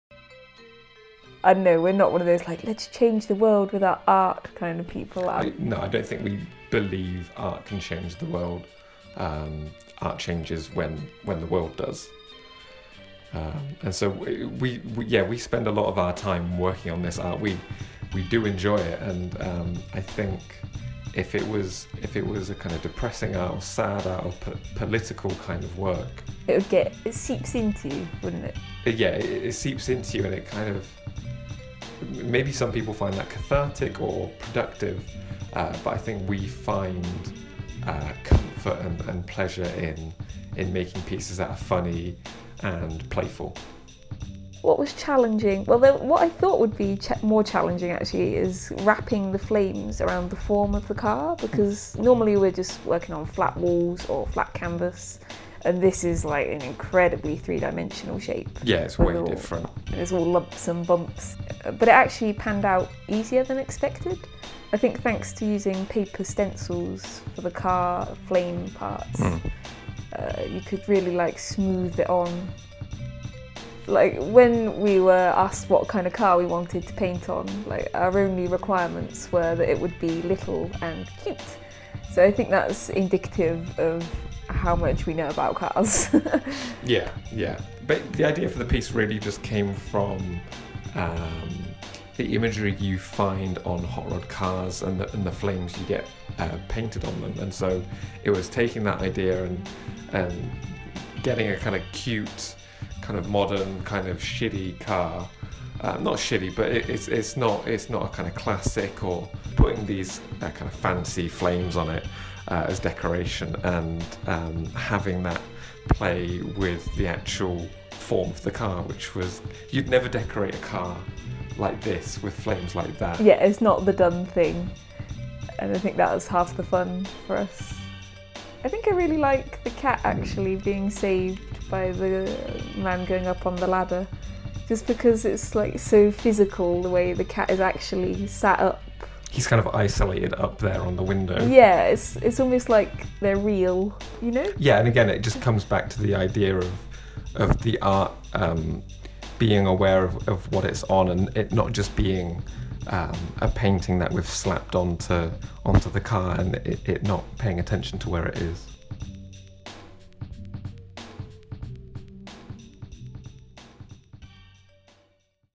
That Escalated Quickly
Hot rod flames are all well and good until they get out of control!

As part of the C4RTI5TS exhibition at the Coventry Transport Museum, on from 15th July to 29th October, 2017!